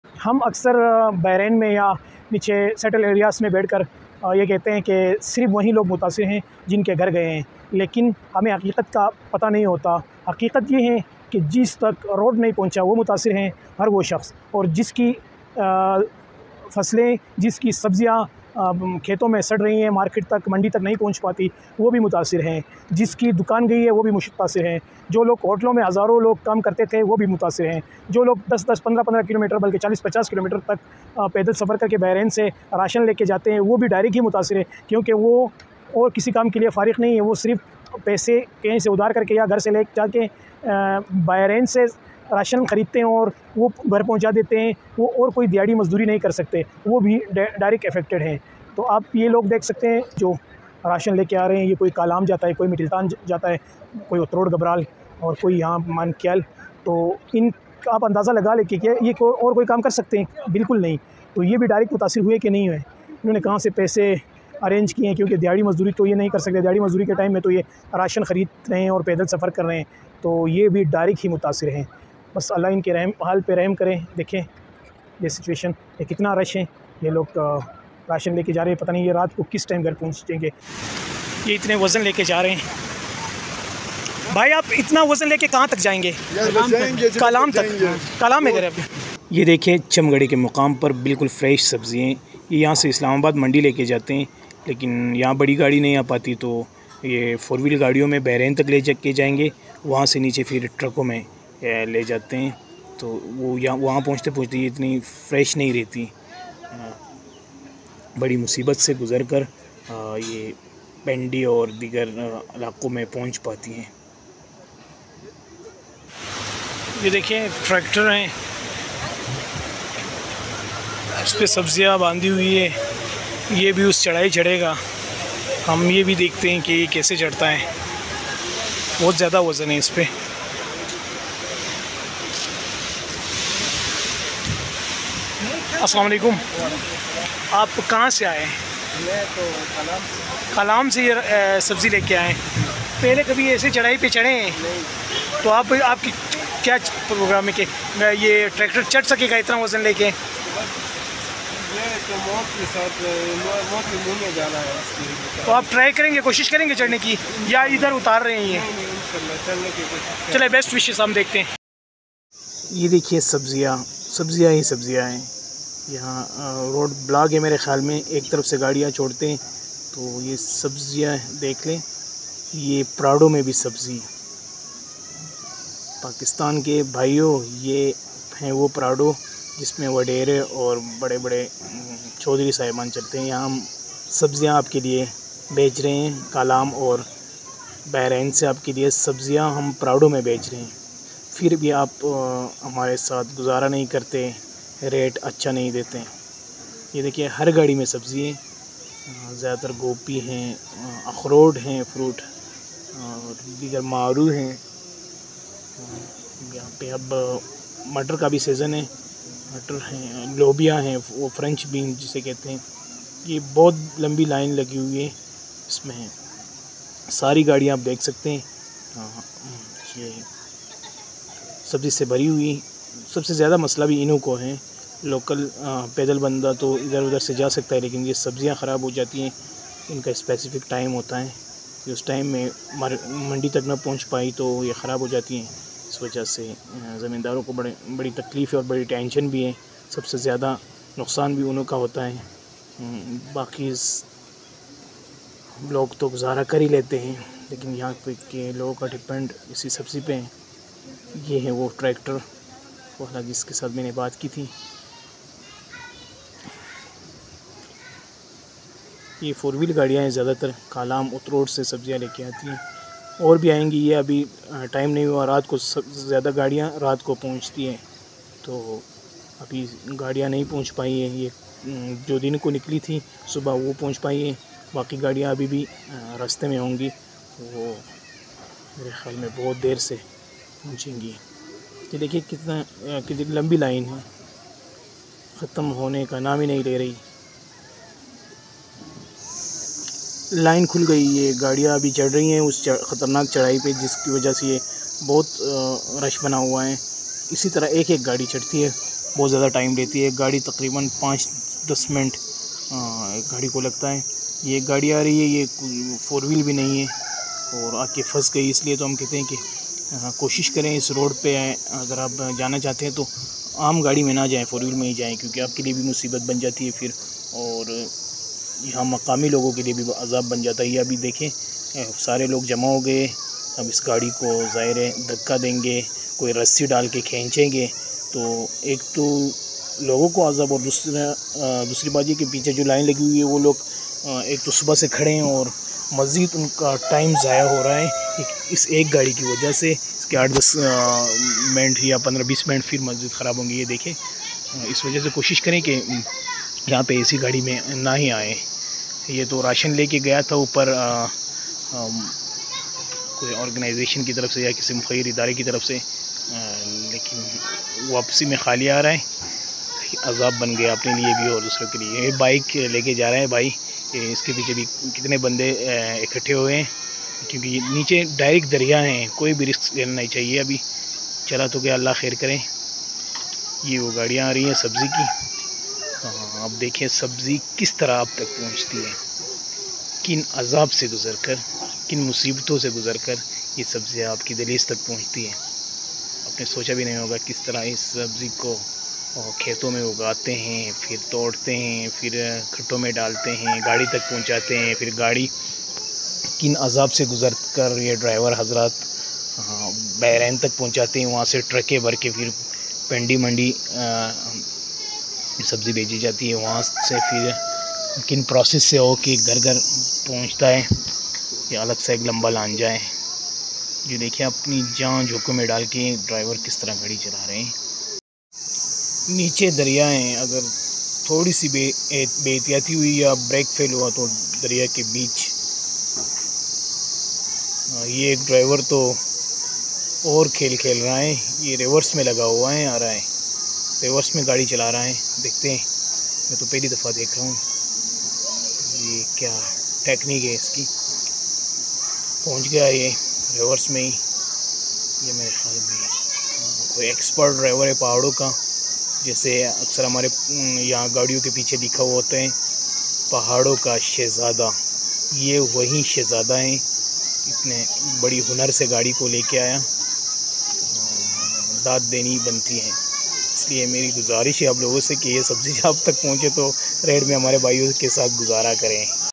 How vegetables are transported from flood affected areas to the market | سبزی منڈی تک کیسے پہنچتی ہے
in this video you can watch the situation which is facing the flood effected areas people of swat Kalam and surrounded areas. how the vegetables are transported from flood affected areas to the market. it's very difficult situation for the formers.